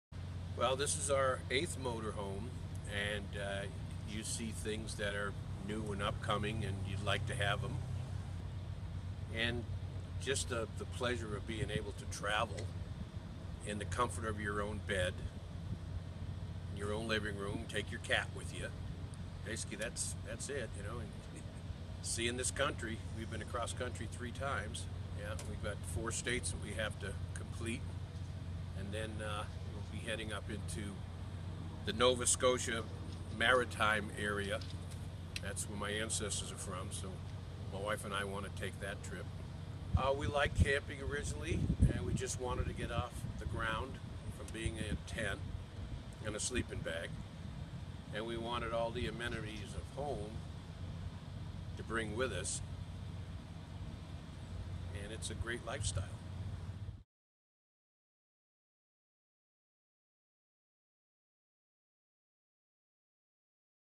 FMCA member talks about his motorhome lifestyle
The FMCA is holding its 90th Family Reunion and Motorhome Showcase at the Deschutes County Fair & Expo Center, where it has held four previous conventions since 2001.

Video by Leilani Rapaport
The Bulletin